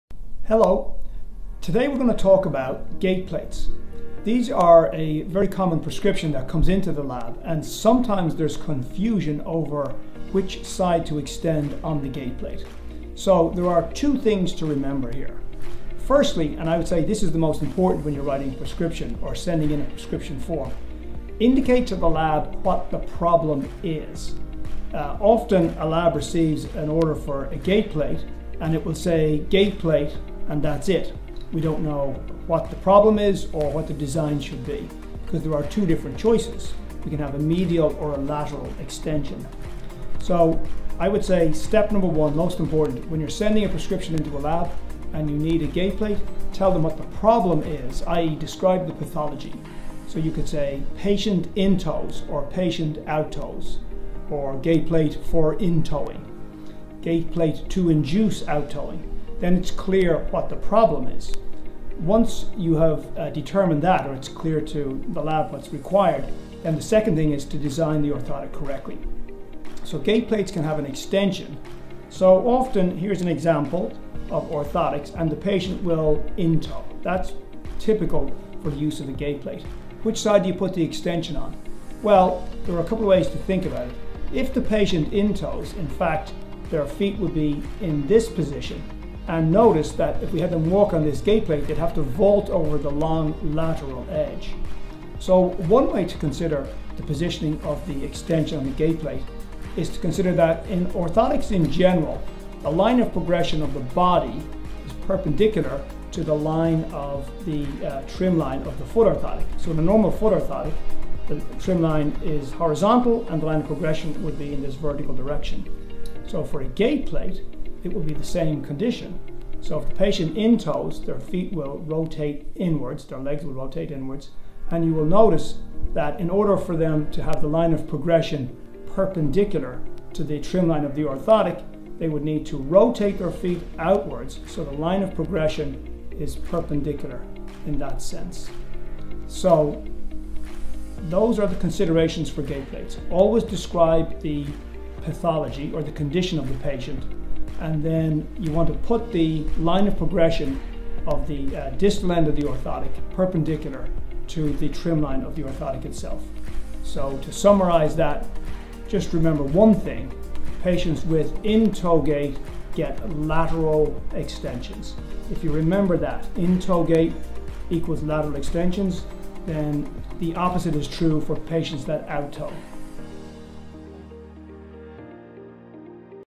Designing Gait Plates for Children
Learn 2 Tips to ensure your Gait Plate designs are correct.